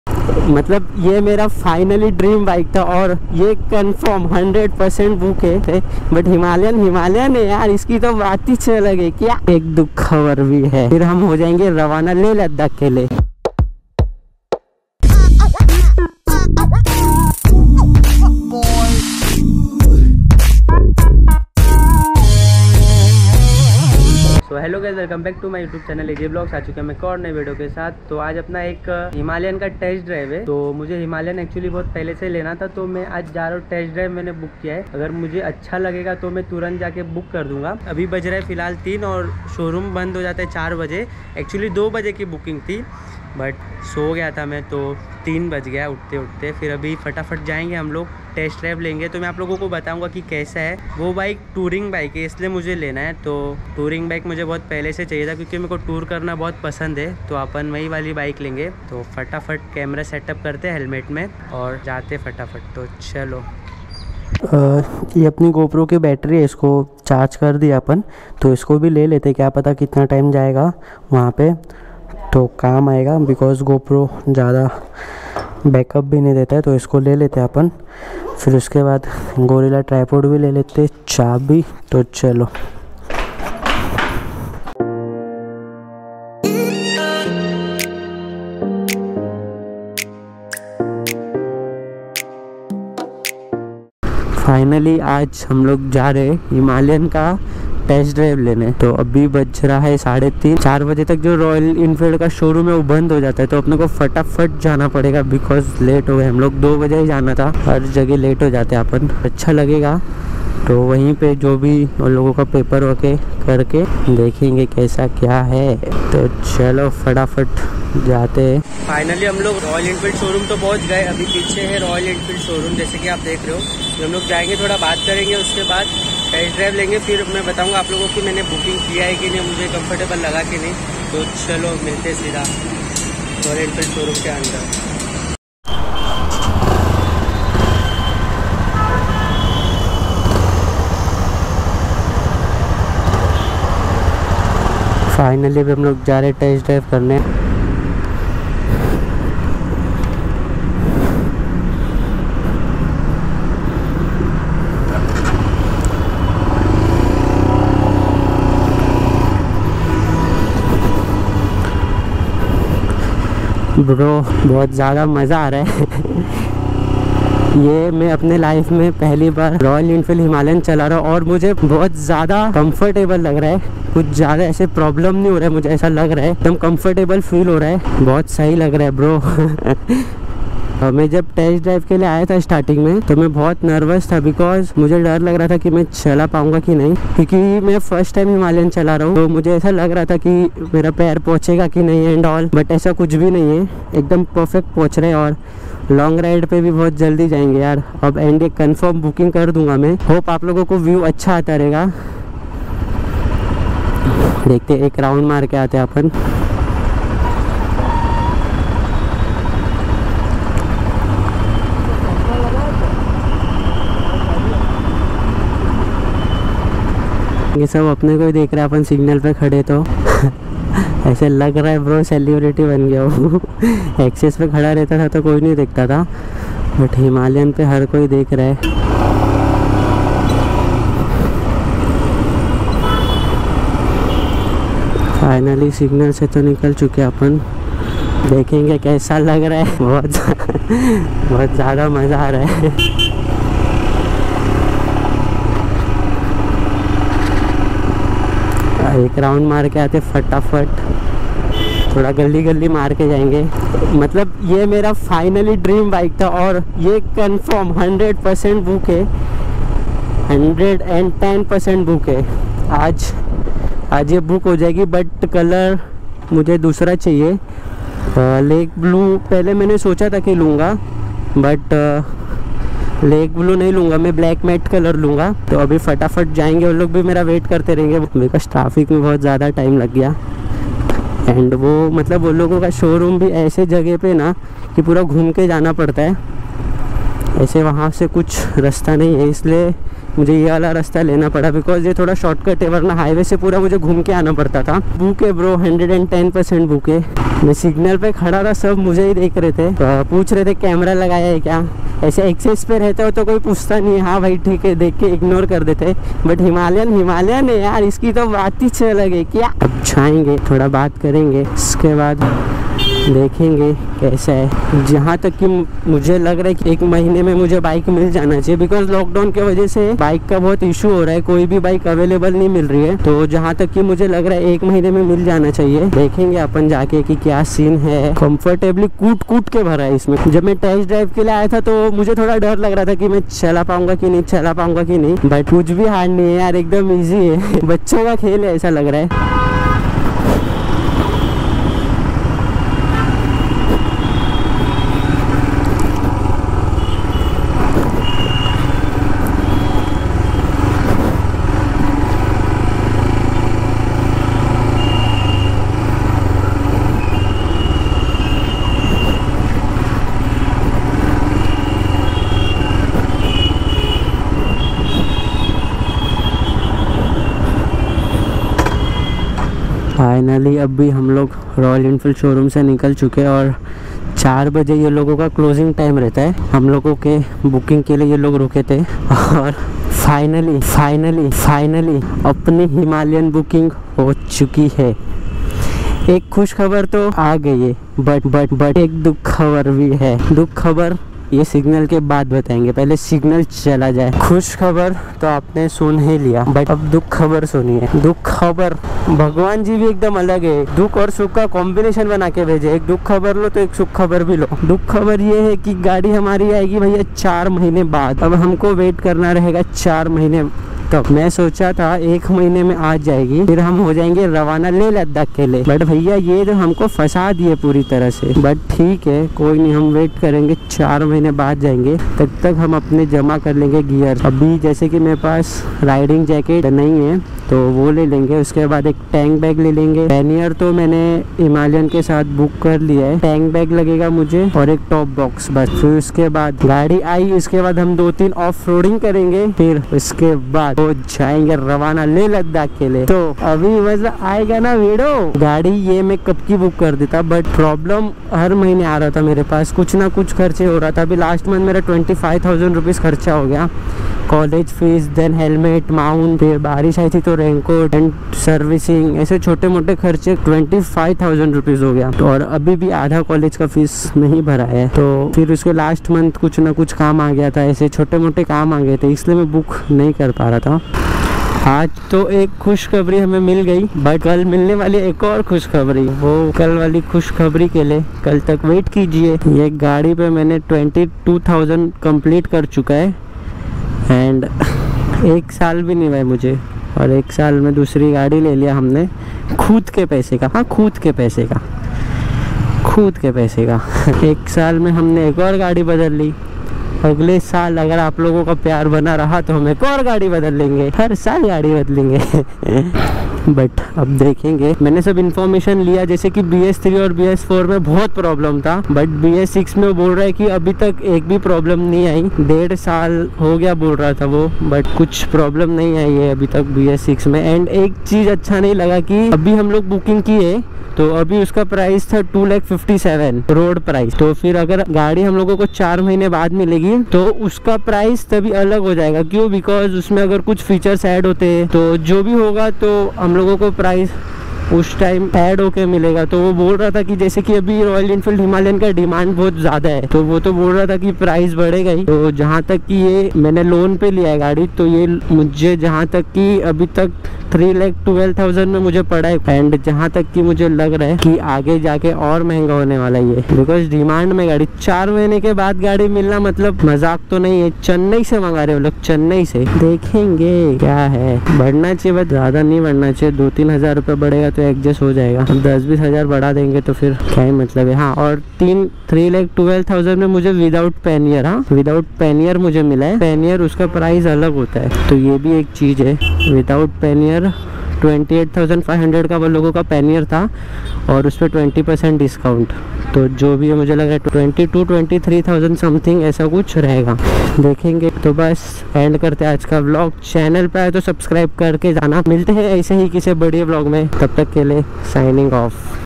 Finally Booked My Dream Bike || RE Himalayan Bs 6 || With My Own Money
Hey Here Aayush Gupta Today booked my new Himalayan Bs 6 With My Own Money its Too much Comfortable .

Camera use

Mount use

Mic use

Mic adapter use

Media mod for GoPro